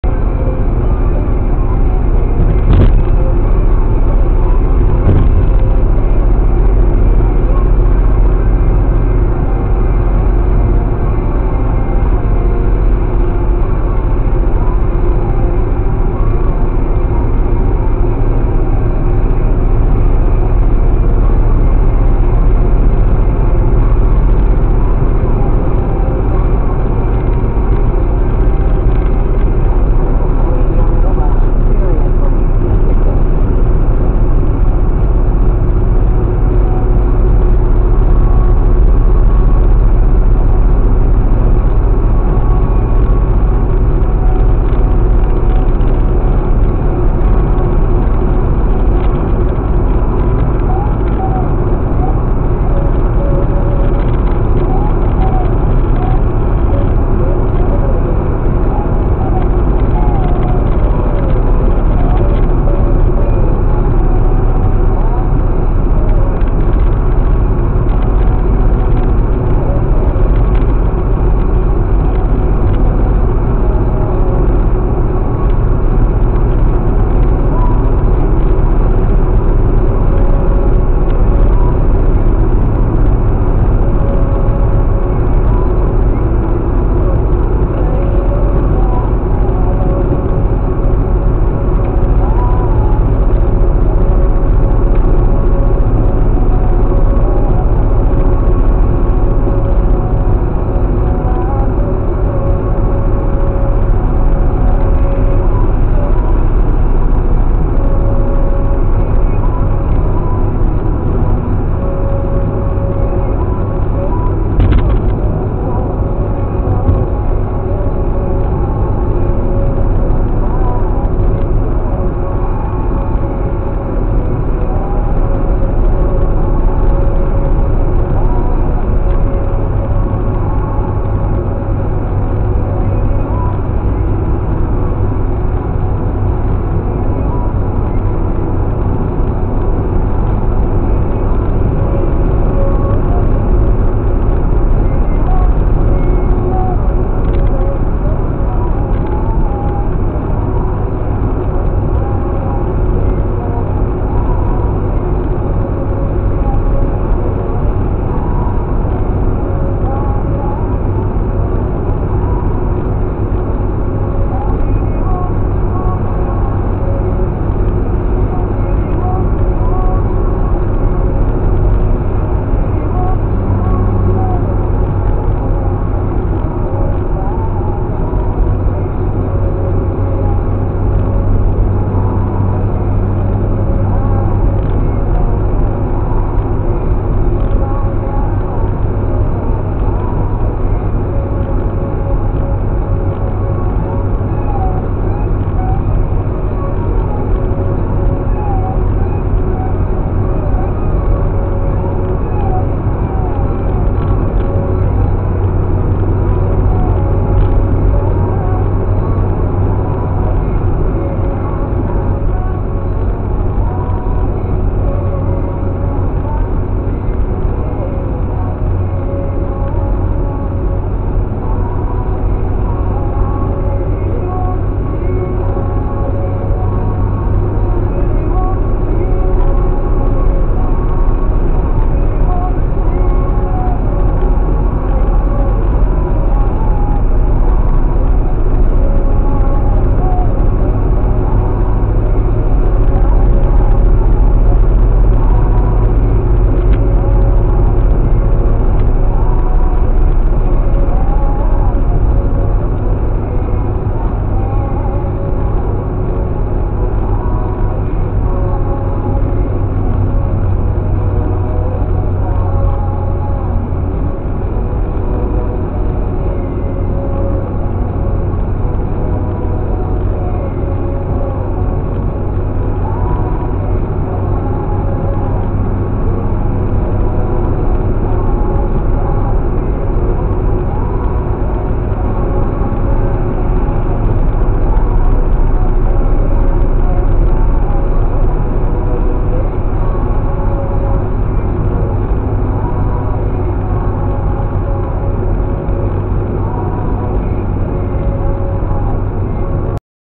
N11 Dublin to Wexford, 1
Nothing amazing happens. No crashes or mad stuff. What I want to show is the beauty of Dublin and it's surroundings - all captured on my dashcam. I hope you will enjoy a pleasant drive in a beautiful city in a beautiful country, Dublin, Ireland. (There may be an occassional trip outside Dublin).